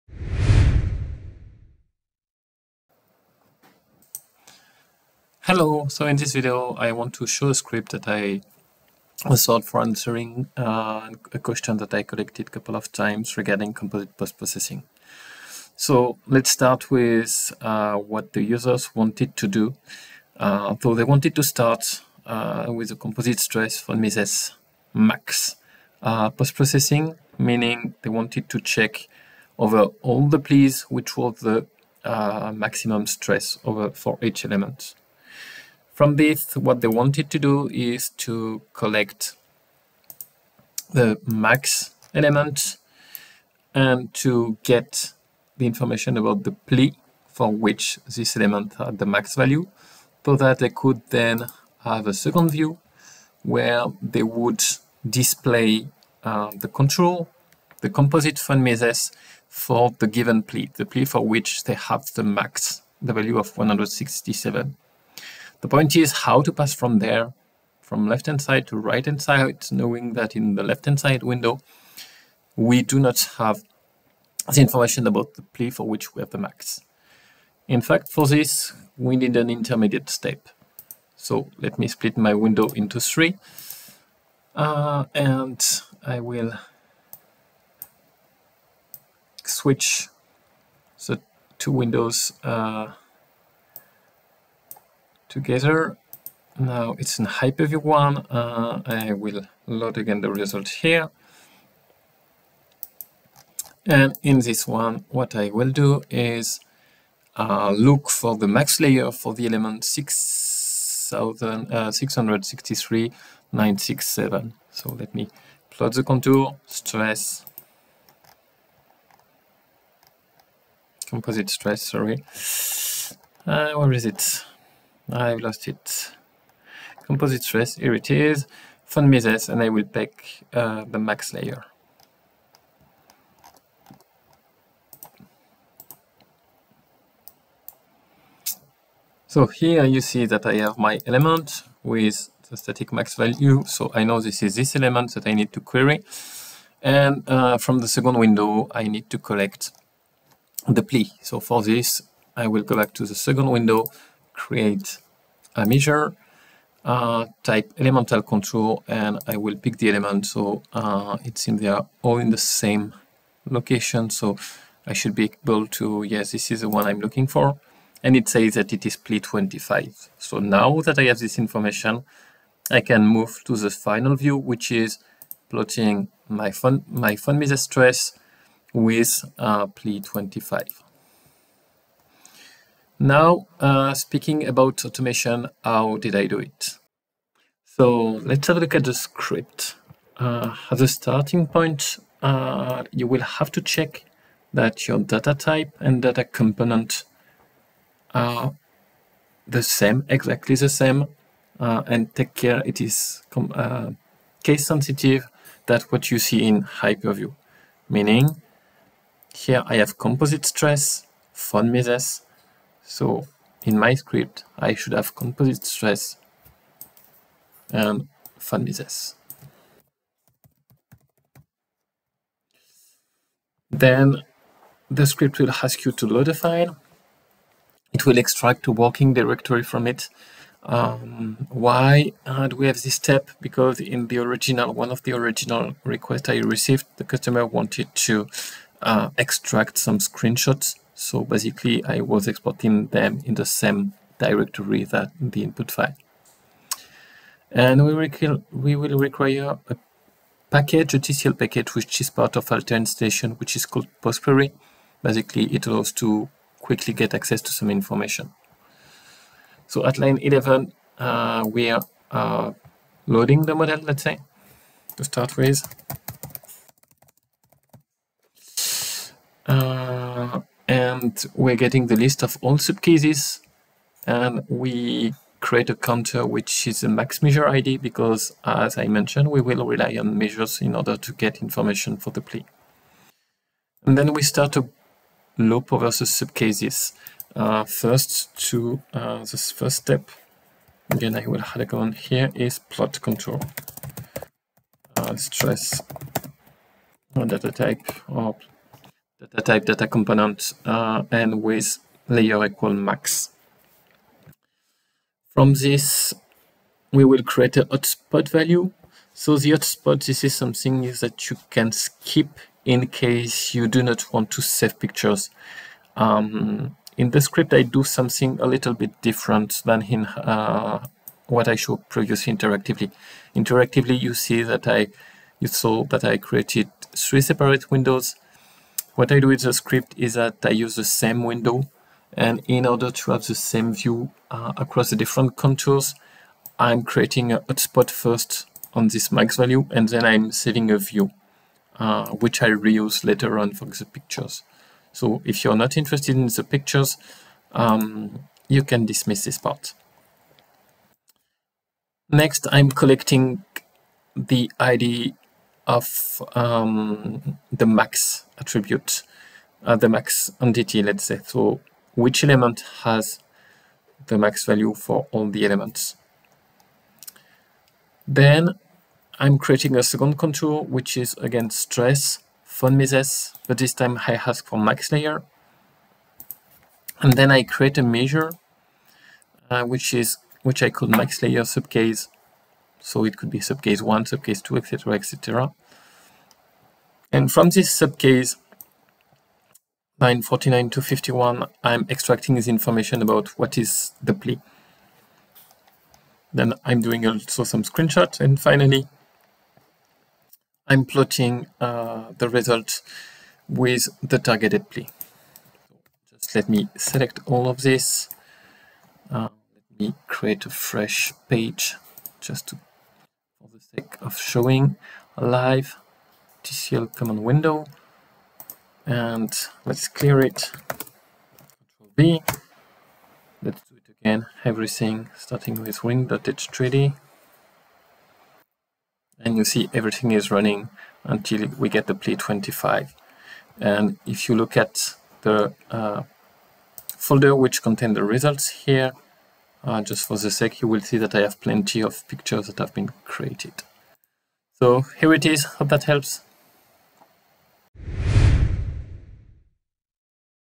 How to display results at ply with peak stress value with HyperView
This video shows how, once you identified the peak stress value, you can plot the stress value at the ply where the max is located. Both the interactive and an automated method are illustrated.